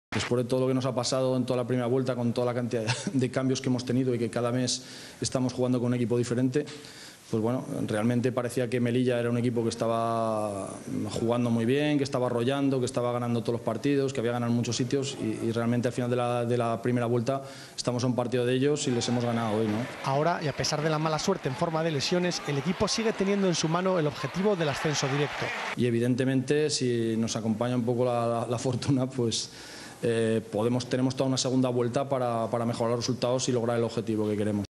Post Partido CAI Zaragoza vs Melilla
3 de enero de 2010, Declaraciones de Jose Luis Abos tras la victoria del CAI Zaragoza ante el líder de la Adecco Oro el Club Melilla Baloncesto en la jornada 17 de la Adecco Oro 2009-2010, última de la primera vuelta. Programa de Aragón Televisión.